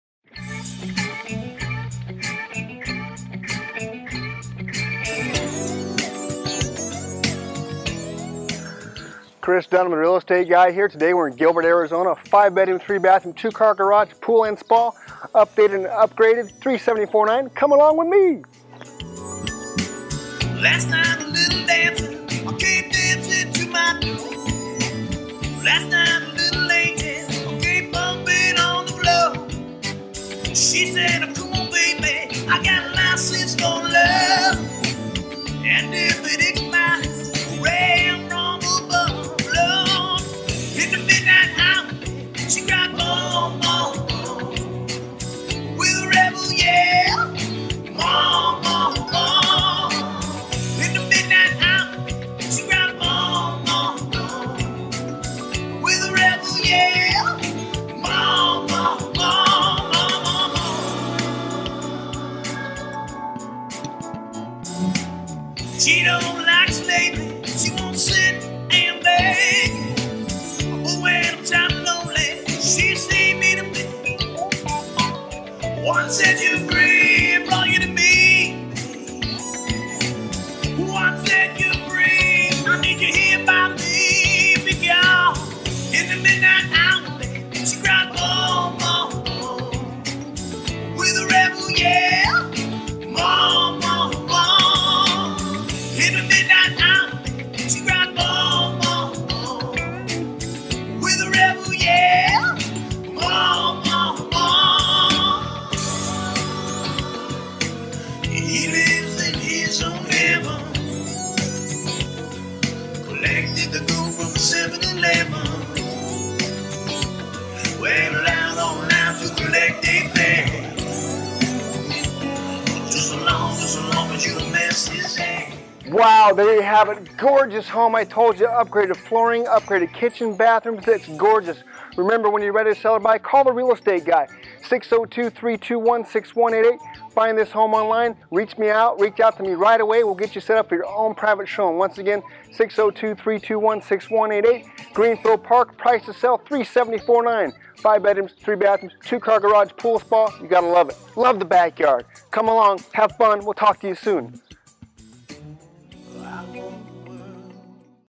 Greenfield Park Home for Sale - 1475 N Roanoke Street
WOW! Only the 3rd Owner!! Honey Stop the Car and Buy this MOVE in READY, Traditional Sale! 5 Large Bedrooms, 3 Full Bathrooms! Updates & Upgrades included the following: Resurfaced (2013) Heated (Propane) Pool/Spa and Deck & Patio (2016). Crown molding in Family Room Downstairs bathroom renovated!

Outside blinds in place for summer months! New AC Units (2) in 2013. kitchen has been updated and upgraded to bring on the WOW Factor! (See Pics). HUGE Breakfast Bar for those morning meals when you're in a rush! They even redid the Laundry Room! Who does that? These folks did and its the bomb diggity! They even built a custom Pergola out back. The Gas Stove runs off Propane. The Drop dead GORGEOUS Tile floors were installed in 2013 (downstairs) and still look brand new!!!

The kitchen features Stainless Steel Appliances. The home was completely painted in the past few years as well. New lighting in the Kitchen, entry and formal dining area. This is a turn key Tri Level. Upon entering you are in the formal living area (perfect for a Game Room-Man Cave) which leads to the formal dining area. The kitchen is next, and its breathtaking to say the least!

Take the stairs down and find yourself in the family room, which is light-bright, open and airy. The downstairs features a HUGE Bedroom adjacent to the full downstairs bathroom (easy to use as a second master down if need be). The downstairs also features bedroom 2 (currently used as a home office). Upstairs features the remaining bedrooms, which are all spacious and ready for your clients to call home!

Great community located just off Greenfield and Baseline. A short distance to "Great Heart Schools" and "The Falls-Event Center". Did I also mention its close to the Freeway? Many, Many restaurants, Dunkin Donuts, Starbucks, Shopping Centers, Retail? It is!!!

This house is a MUST SEE!! (Seriously, I am not just saying this, it really is a super nice home) owners moving back home to Oklahoma, give us a great price and we'll throw in the Washer & Dryer, maybe even the Stainless Steel Fridge! Oh my, I almost forgot about the easy access to the walking, biking and running trail directly behind the home, so you'll NEVER have neighbors behind you!! If you have an active lifestyle, love to Run, Walk or Bike. bam!

Added bonus for you! Last but not least, the BACK YARD!!! WOW! This place is a paradise for you to enjoy day in and day out. The backyard OASIS, just takes your future HOME up a NOTCH!

Arrange your private showing by calling me today;
Chris Dunham
"The Real Estate Guy!"®